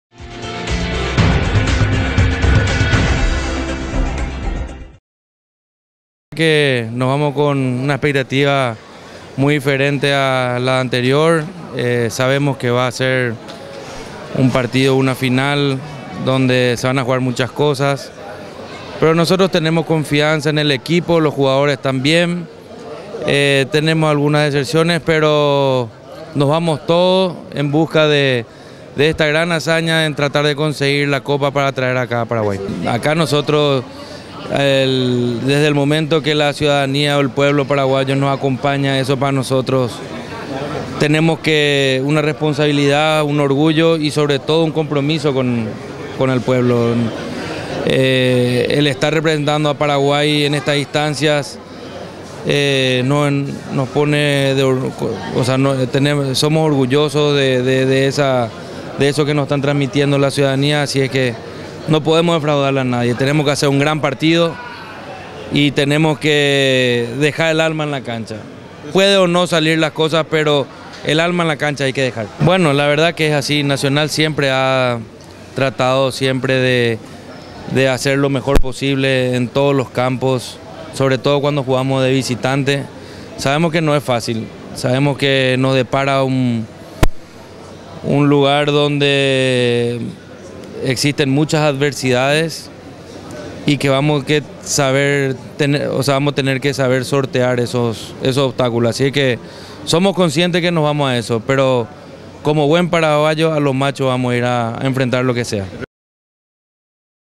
Harrison: "Vamos a ir a enfrentar a lo macho"
El presidente tricolor anuncia que pelearán hasta el último minuto para quedarse con el título de la Copa Libertadores de América. Cargados de ilusión, viajó el plantel en la tarde de este lunes rumbo a Buenos Aires para la revancha ante San Lorenzo de Almagro.